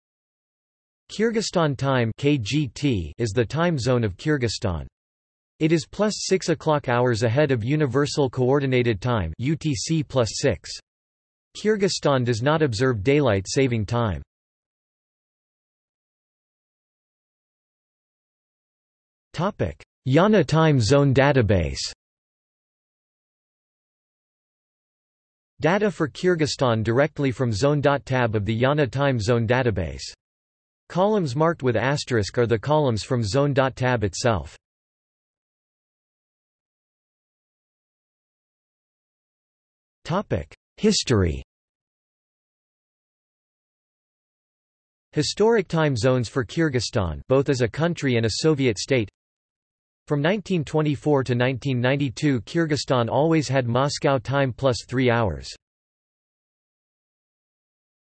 Time in Kyrgyzstan | Wikipedia audio article
This is an audio version of the Wikipedia Article:
Time in Kyrgyzstan

00:00:15 undefined
00:00:36 undefined

- Socrates

SUMMARY
Kyrgyzstan Time (KGT)is the time zone of Kyrgyzstan. It is +6:00 hrs ahead of Universal Coordinated Time (UTC+6). Kyrgyzstan does not observe daylight saving time.